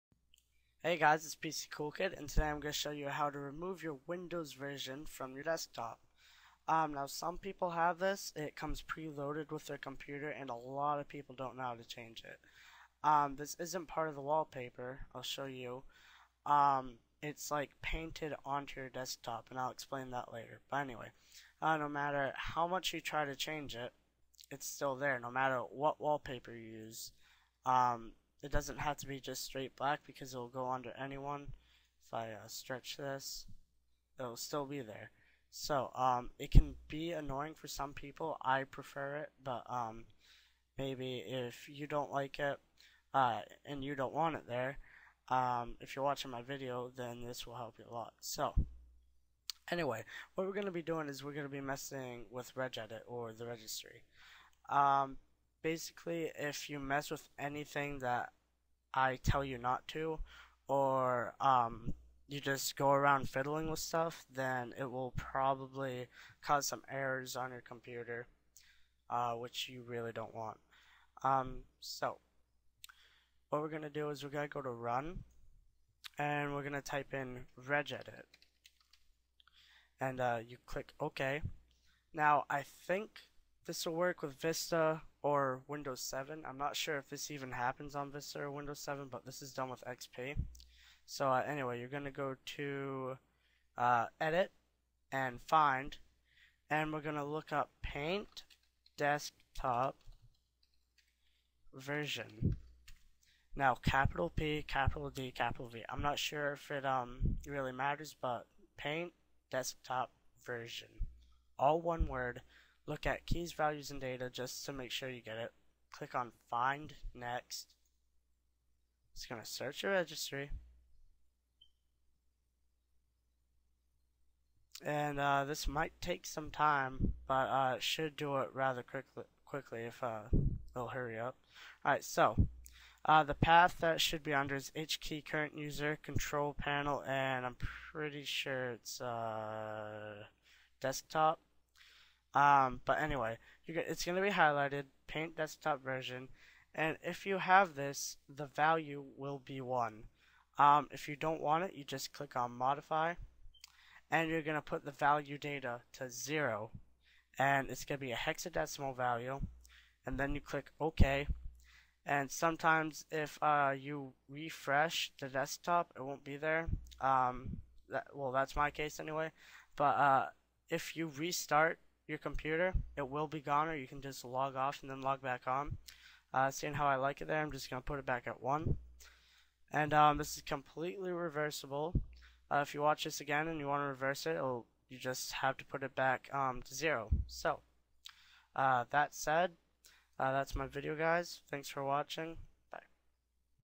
Tweak: Remove Windows Version From Wallpaper (HD)
Hey guys, in this video I show you how to remove your Windows Version from your desktop. It is sometimes pre-loaded on some computers and laptops in order to verify your version of Windows, or at least I think.